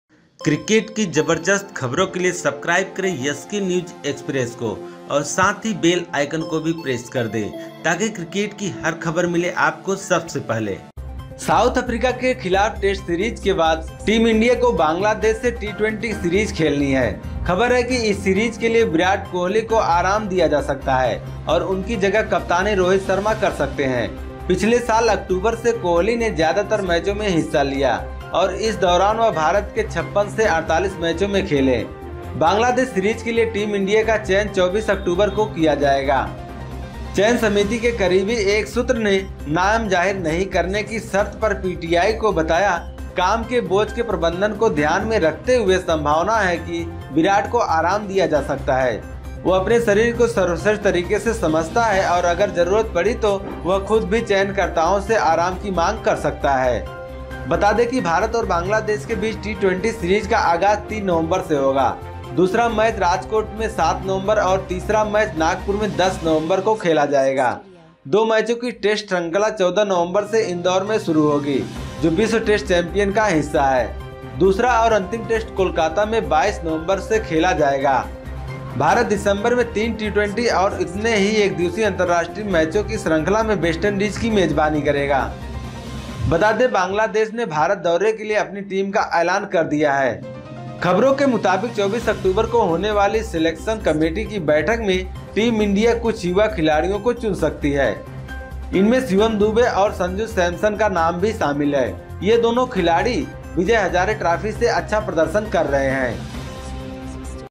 बांग्लादेश के खिलाफ टी20 सीरीज में रोहित शर्मा कर सकते हैं कप्तानी, जानिए ये है वजह
साउथ अफ्रीका के खिलाफ टेस्ट सीरीज के बाद टीम इंडिया को बांग्लादेश से टी20 सीरीज खेलनी है. खबर है कि इस सीरीज के लिए विराट कोहली  को आराम दिया जा सकता है और उनकी जगह कप्तानी रोहित शर्मा कर सकते हैं. पिछले साल अक्टूबर से कोहली ने ज्यादातर मैचों में हिस्सा लिया और इस दौरान वह भारत के 56 में से 48 मैचों में खेले. बांग्लादेश सीरीज के लिए टीम का चयन 24 अक्टूबर को किया जाएगा.